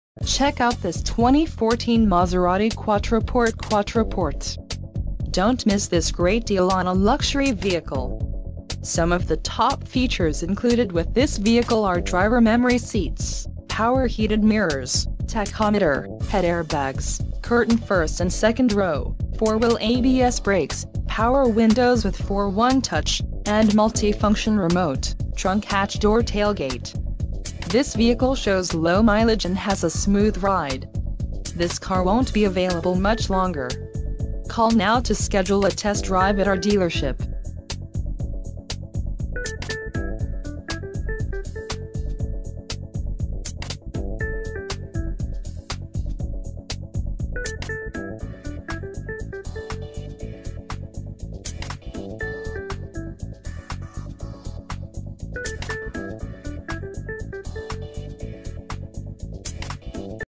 2014 Maserati Quattroporte QUATTROPORTE - Foreign Cars It...
All Wheel Drive Quattroporte S Q4 BIANCO on NERO POLTRONA FRAU LEATHER INTERIOR, QUATTROPORTE ALLWHEELDRIVE MASERATICHARLOTTEThis 2014 Maserati Quattroporte AWD is equipped with a 3.0L Twin Turbo V6 Engine and produces 404hp with 406Lb of Ft Torque & 8 Speed Automatic Transmission with Skyhook Controlled Suspension. Equipment Consist of a Keyless Entry with Remote Start, Dual Stage Airbag System, Power Sunroof, Featured with Adaptive Bi-Xenon Headlights, All Leather Nero Seats, Steering Wheel and Front to Rear Parking Sensors, Also Auto Dimming Side Mirrors. Its has it Upgraded 8.4" Maserati Touch Control & Navigation Screen with an 600w 10 speaker Premium Sound System. Also as Standard Features; Sport Exhaust Twin Dual Tailpipes, Power rear Side Window Sunshades.